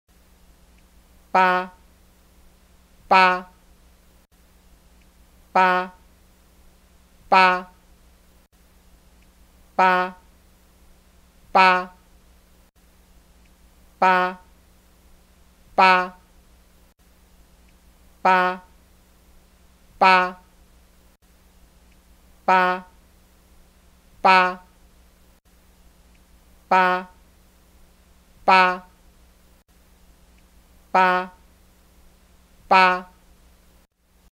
How to Say 'Eight (8)' in Chinese
Learn how to correctly say 'Eight (8)' (八/八/Bā) in Mandarin Chinese.